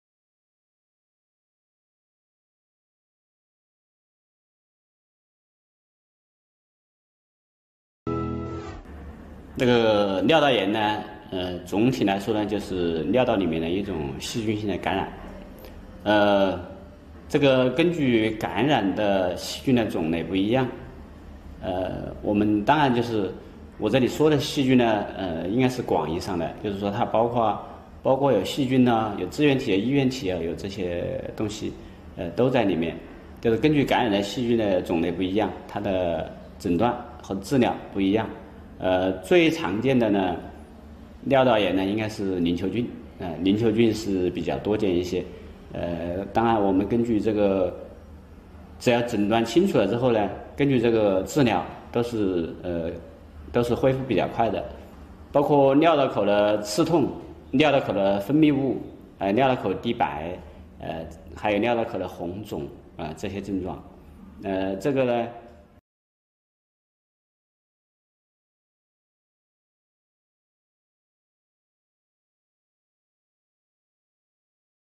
什么是尿道炎 杨向利 武汉科技大学附属天佑医院
尿道炎总体就是尿道里面的一种细菌性感染，根据感染的细菌的种类不一样，诊断和治疗不一样，最常见的尿道炎是淋球菌，根据诊断清楚了之后治疗都是恢复比较快的，包括尿道口的刺痛、尿道口的分泌物、尿道口滴白，还有尿道口的红肿这些症状，如果出现了这种情况一定要注意个人卫生，还有自己家人的卫生情况，因为尿道炎有些是可以出现交叉感染的。

What is urethritis
Urethritis is generally a bacterial infection in the urethra, Depending on the type of bacteria infected, Diagnosis and treatment are different, The most common form of urethritis is gonococcus, According to the diagnosis, the treatment is relatively fast, It includes tingling of the urethra, secretion of the urethra, and white dripping of the urethra, And the swelling of the urethra, If this happens, we must pay attention to personal hygiene, And the health of my family, Because urethritis some can appear cross infection.